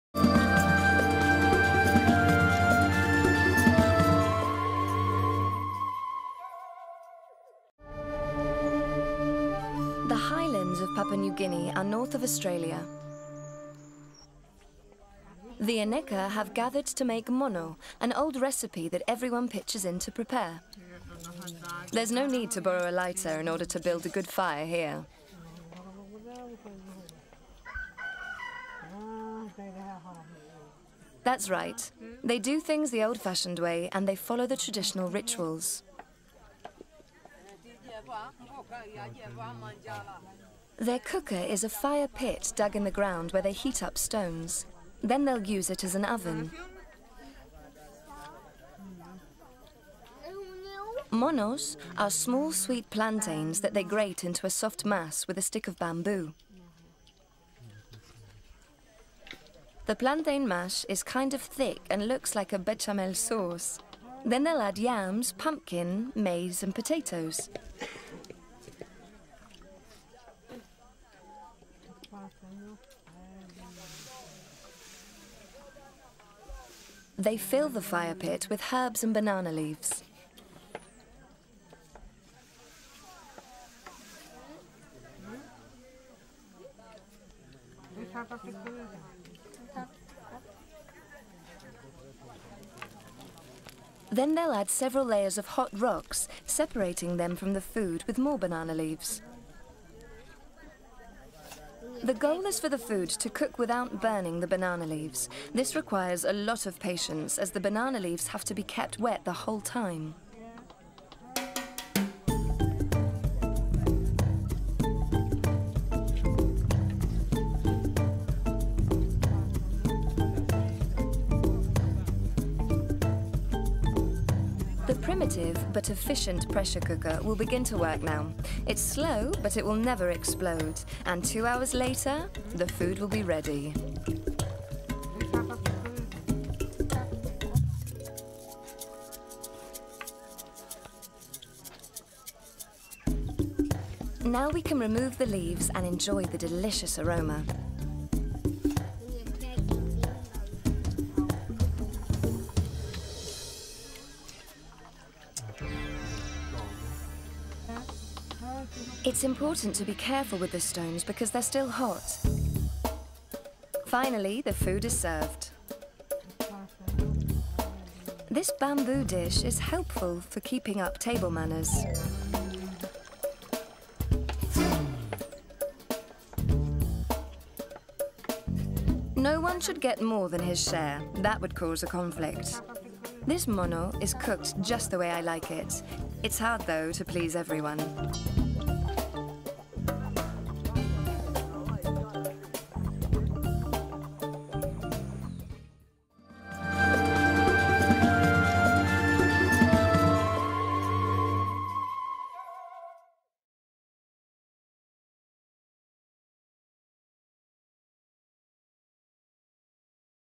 What are the Highlands? We analyze this culinary curiosity located in Papua New Guinea
In Australia, (specifically Papua New Guinea), the way of feeding is tremendously curious. In fact, we prove it by talking to you about the Highlands. This is how they make it to be able to eat it, with a dough that when made is a kind of béchamel, somewhat thick. Discover everything in this report.